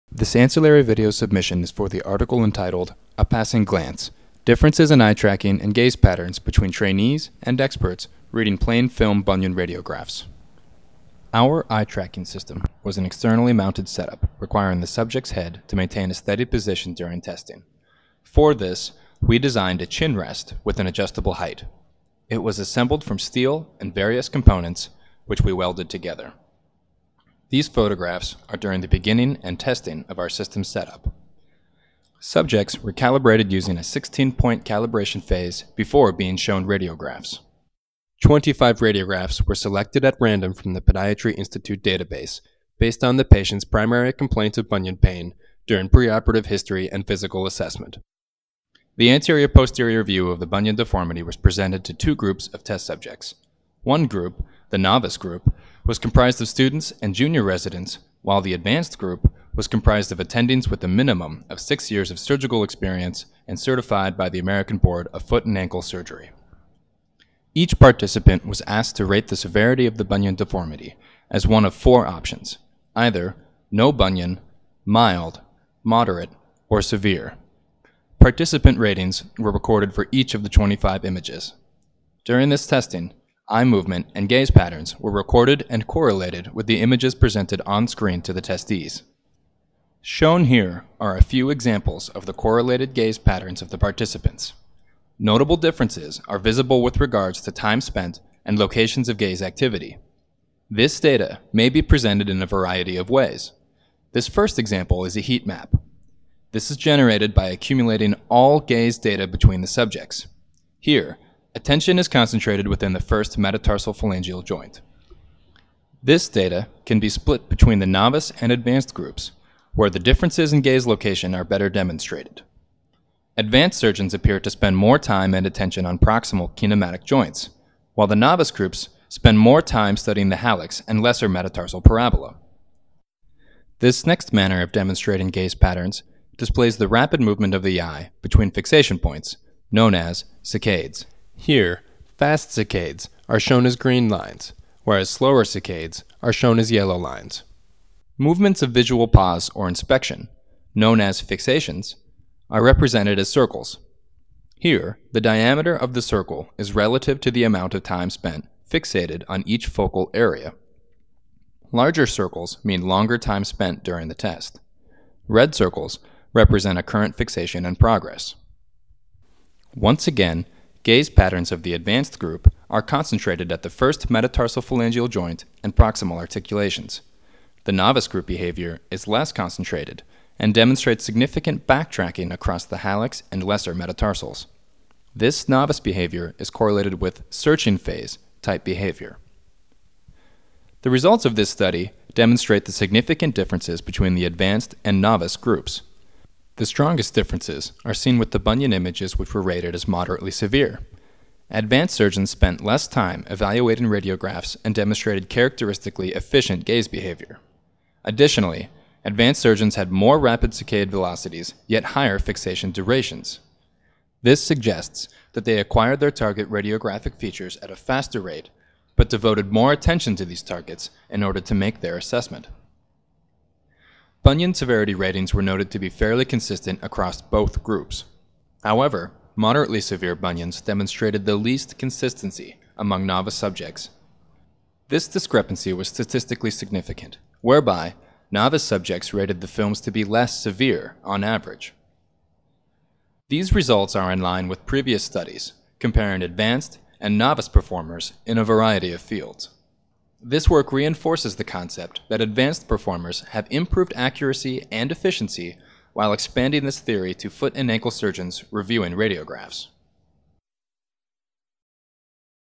Eye tracking data and Bunion surgery
Eye tracking and gaze pattern studies have been used to evaluate human behavior for decades. This is because of its ability to reveal conscious and subconscious behaviors when subjects are tasked with observation, decision making, and surgical performance. Many have popularized the use of this technology for radiographic assessment while evaluating radiologist behaviors, but little has been described for surgeon behavior patterns when evaluating preoperative deformities by radiograph. Because the radiographic assessment strongly influences surgical selection, the present study was designed to evaluate the differences between groups of novice and experienced surgeons' gaze patterns when tasked to describe hallux valgus deformities. The subjects were asked to rate the deformity as “none,” “mild,” “moderate,” or “severe.” Using an externally mounted eye tracking system, our study assessed saccades, fixations, overall time spent per radiograph, and the subjects' chosen bunion rating. Both the novice and advanced groups of foot and ankle surgeons were tasked to evaluate 25 total anteroposterior radiographs from patients who presented with a primary complaint of bunion pain. These patients were chosen at random, such that all participating surgeons had no previous patient familiarization. Statistically significant differences were observed with regard to the activity and rating of the moderate bunion films. The experience of surgeons does appear to modify gaze behavior with respect to time and attention, such that less overall time spent per image is needed by the advanced group, with improved efficiency. Future academic curriculum and training techniques could be developed to reflect these potential technical differences in search behavior, diagnostic technique, and surgical selection strategy.